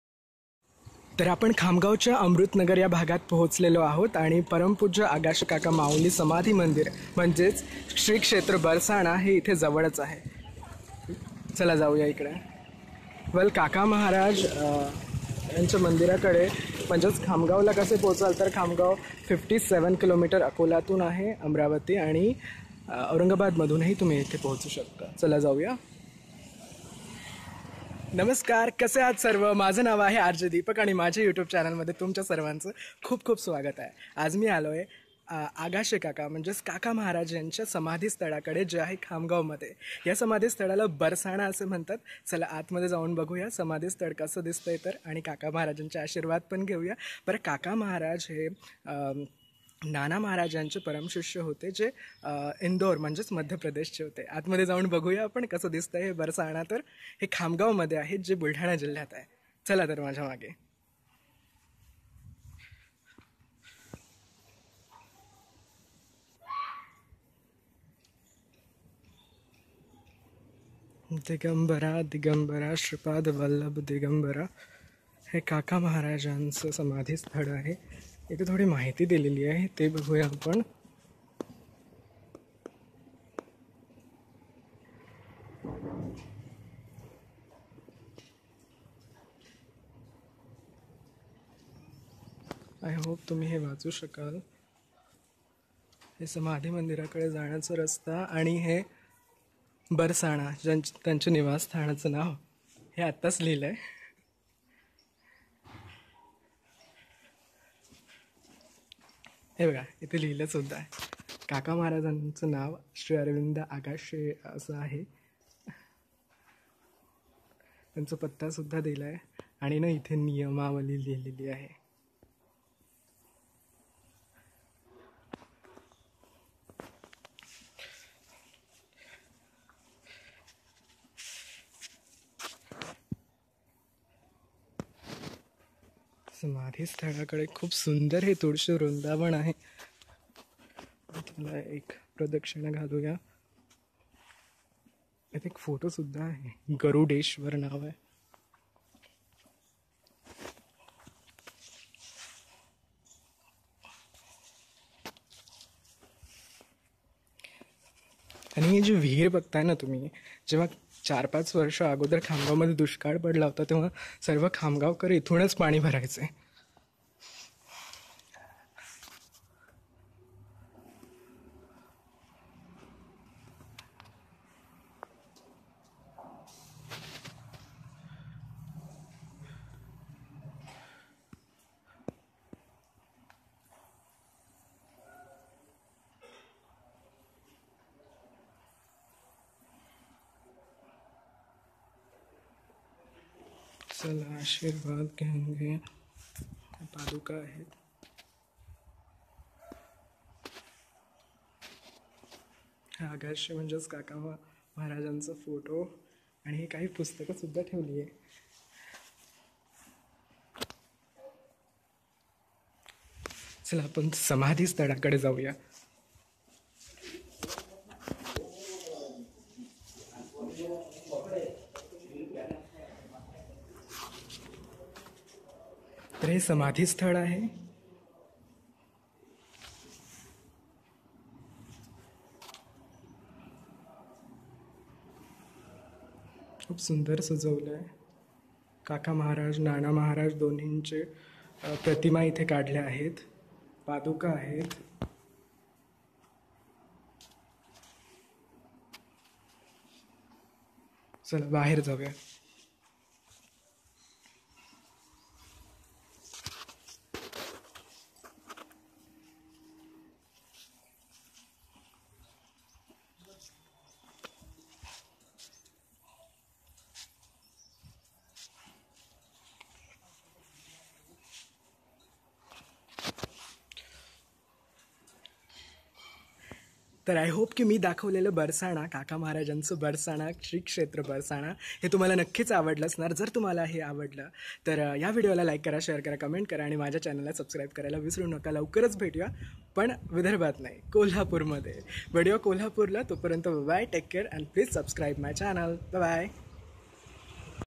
श्री काका महाराज समाधी मंदिर खामगांव | Kaka Maharaj Samadhi Mandir | Khamgaon | Buldhana | RJ Dipak
Kaka Maharaj alias Arvind Agashe Kaka Mauli of Khamgaon Dist Buldhana is one of the Spiritual Saint in Datta Sampradaya of Hinduism. He is the disciple of Nana Maharaj Of Indore (MP).
Who Was the prominent Saint in Hinduism during 20th Century (Gurucharita tradition of Dattatreya). Shri Nana Maharaj was the direct disciple of Shri Vasudevanand Saraswati Swami Maharaj (Tembe Swami)